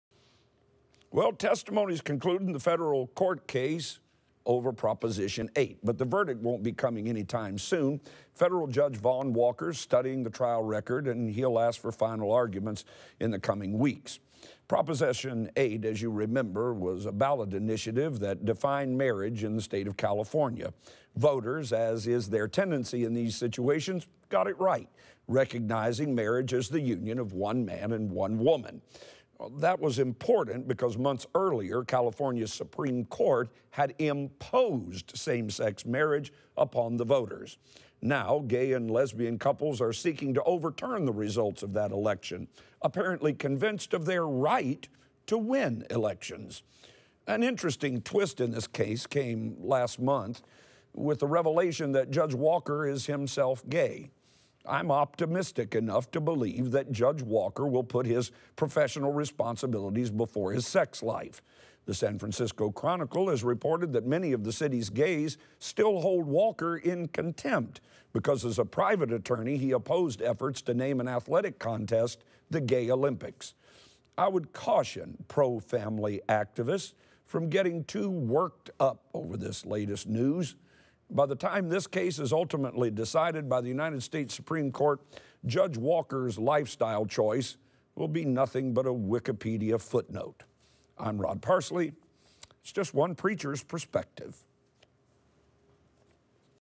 Perspective: Marriage on Trial in California
Pastor Rod Parsley examines current events from a biblical perspective.